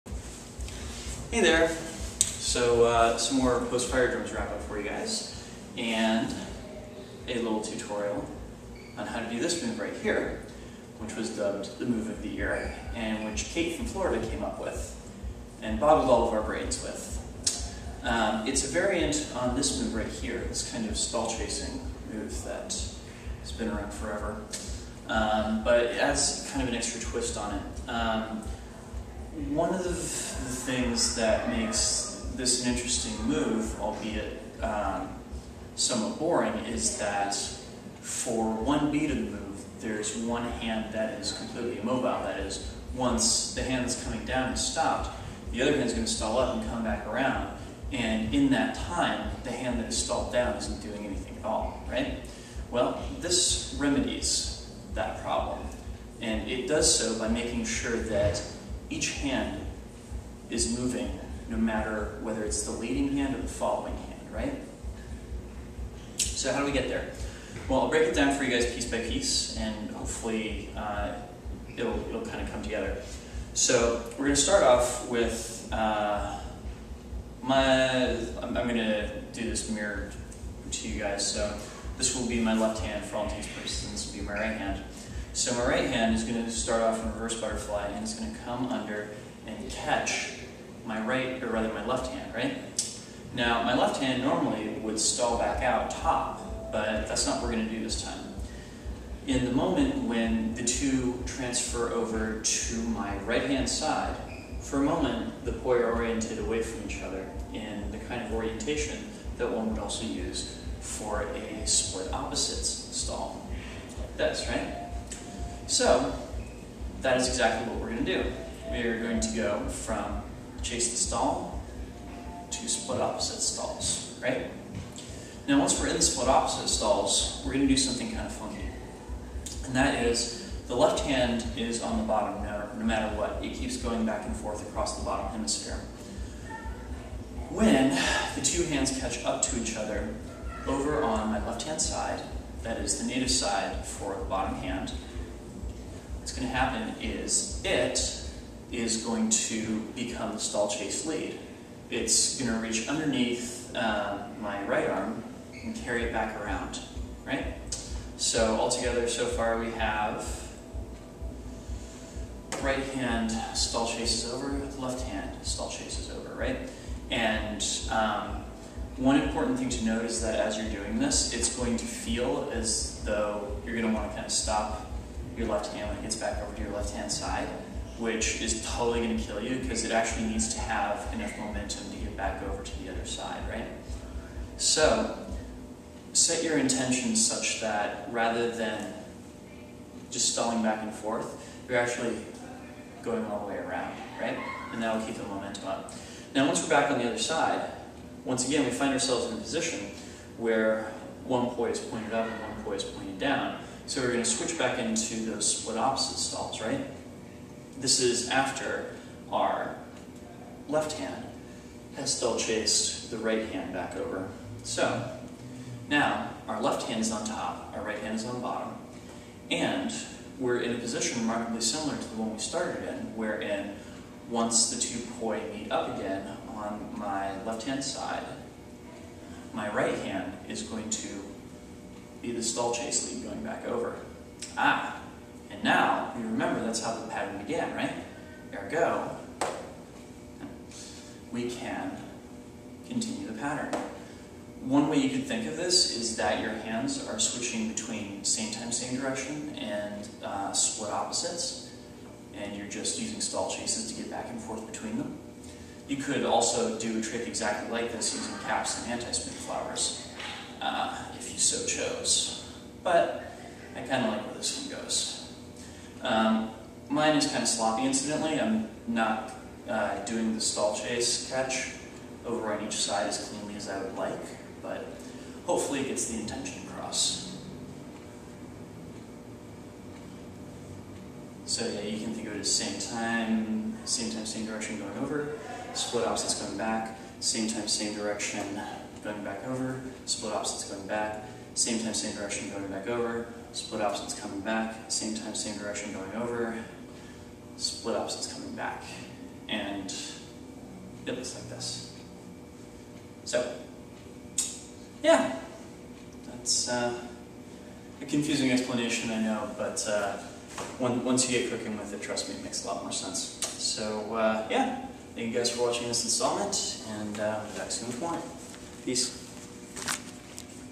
Drex's Tech Poi Blog #88: Stall chases with split-time opposites stalls
Another move from this year's Firedrums. Kate from Florida came up with this one (thought it actually appears that Mel first demoed it in his "Me and my shadow" vid last year), that takes the old stall chase exercise and adds a twist to it by switching between the stall chase and a split-time opposites stall. It reminds me a bit of another variant on the stall chasing that Mel does, but has a rather unique flair that I dig. Enjoy! :)